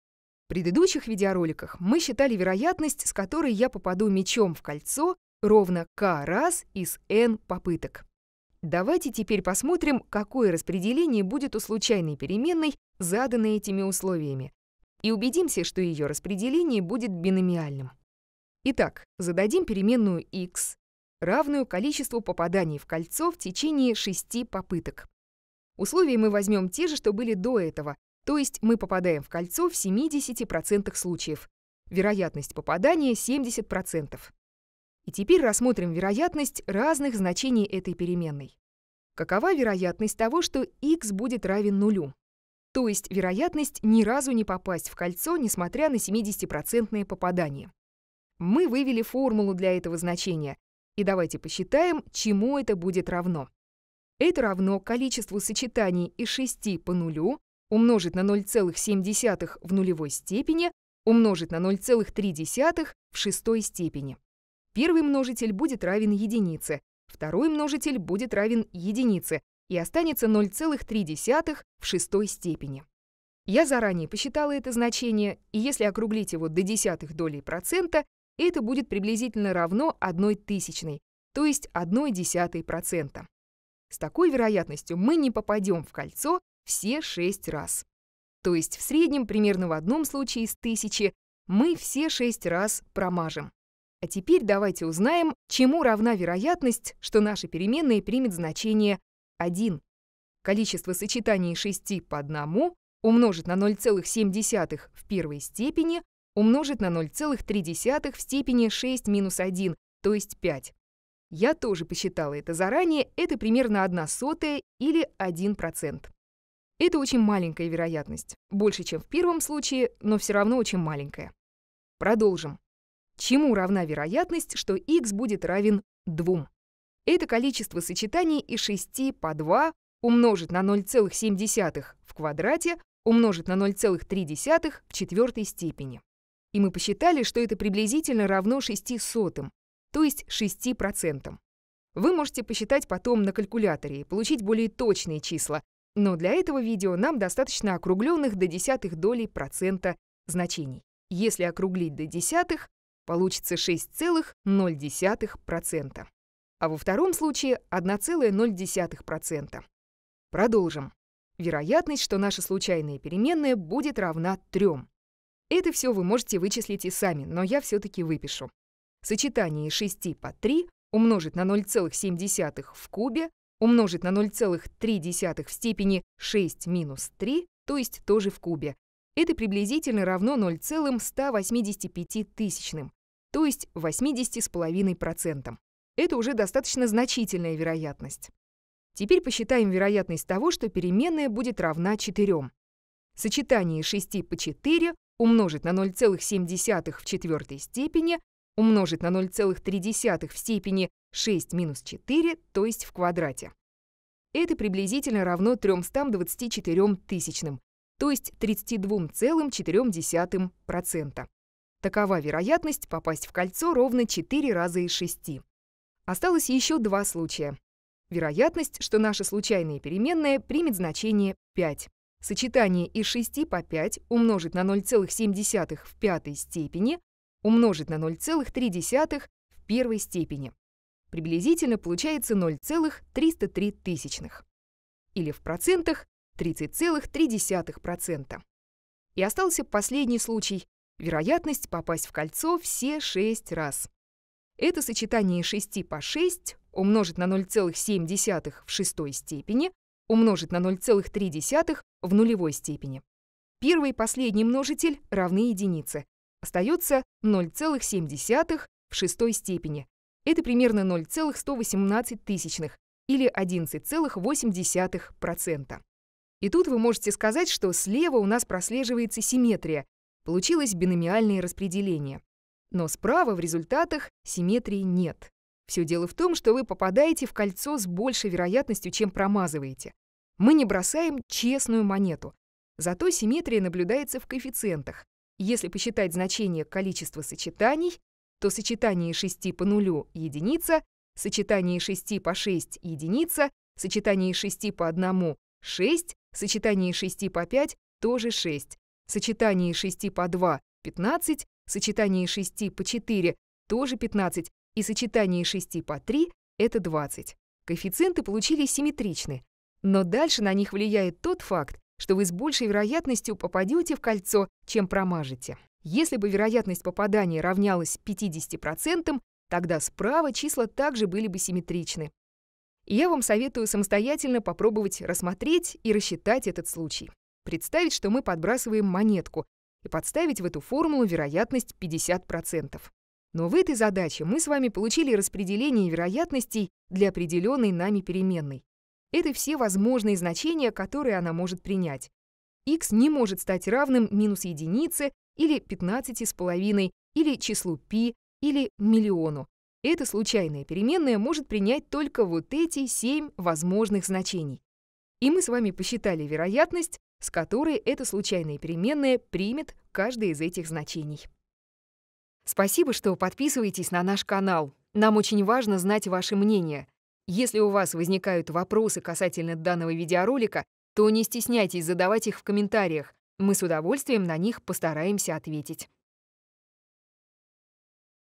Биномиальное распределение вероятностей (видео 60) | Статистика и теория вероятностей
В этом видео мы рассматриваем распределение случайной переменной k, равной количеству попаданий мячом в кольцо за n попыток, и ищем в ней симметрию.

Это видео - русская версия видео «Free throw binomial probability distribution» Академии Хана This video is a Russian dubbed version of the Khan Academy video "Free throw binomial probability distribution"
Перевод был выполнен при поддержке Oxford Russia Fund.